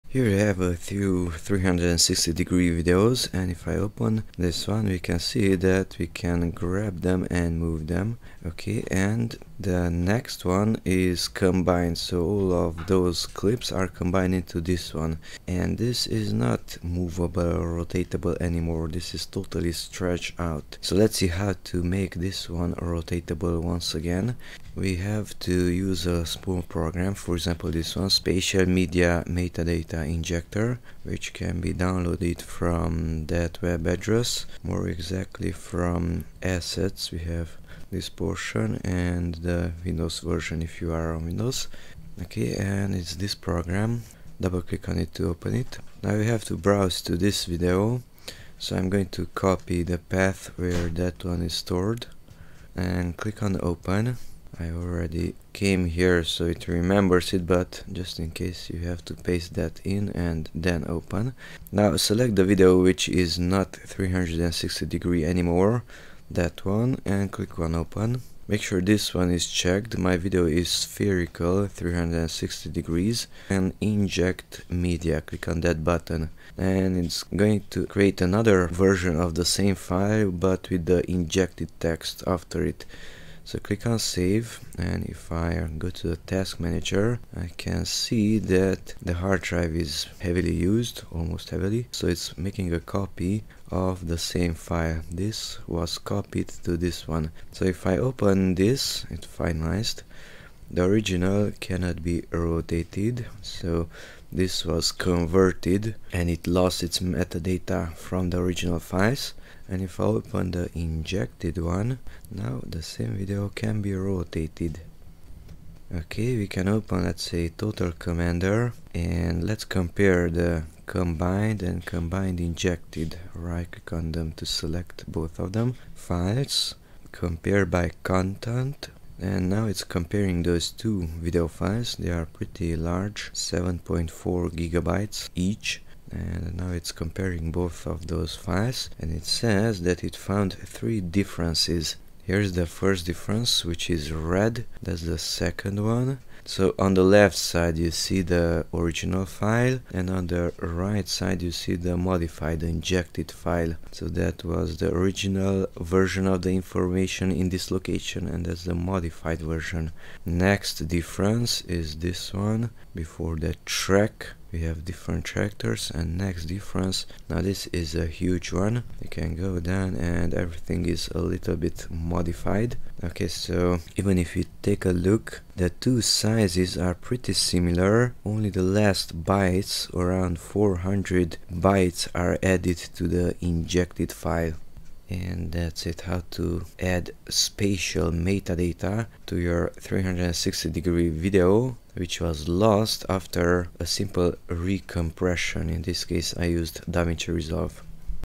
How to add 360° data to MP4 (Spatial Media Metadata Injector, VLC player, Insta360 One X2)
At Assets click on "360.Video.Metadata.Tool.win.zip"

Recompressing a 360° video will make it lose the 360 related metadata, and thus VLC player under Windows isn't able to rotate the video, but VR 360 players on smartphone still play the video correctly and you're able to see in 360°.